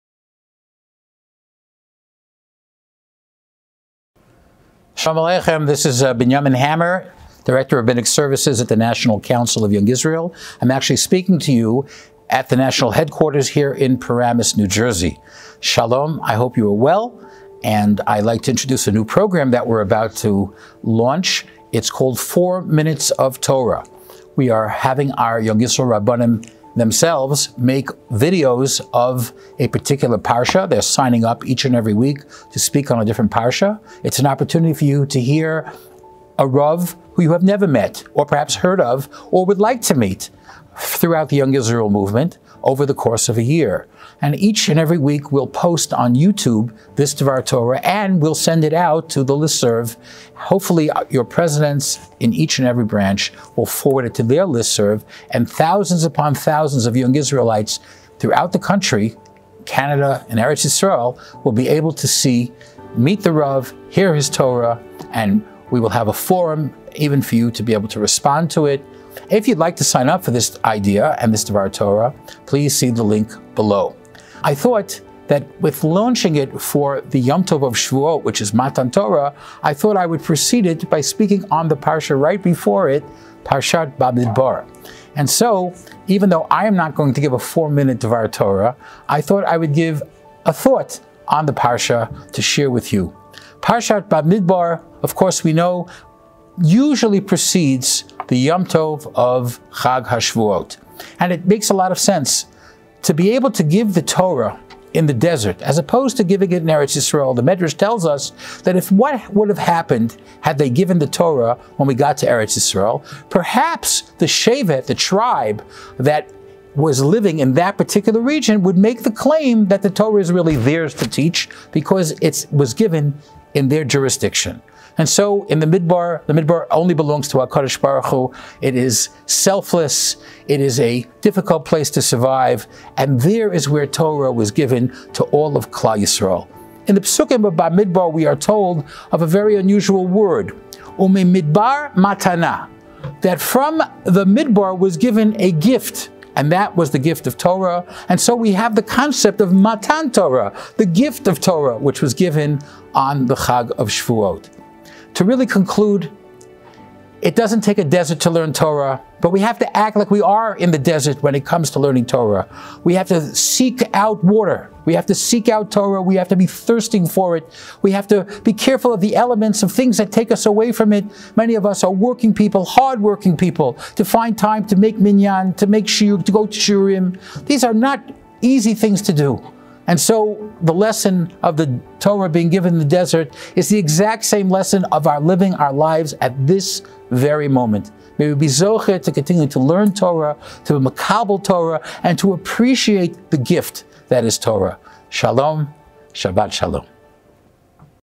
Introduction to 4 Minutes of Torah from Rabbi Binyamin Hammer
The National Council of Young Israel is proud to present a new series from our Department of Synagogue Services called "4 Minutes of Torah."

Each week the National Council will send out a video from one of our branch Rabbanim with a short 4 minute D’var Torah on the Parsha, which will be sent out to our over 20 thousand Young Israelites each week.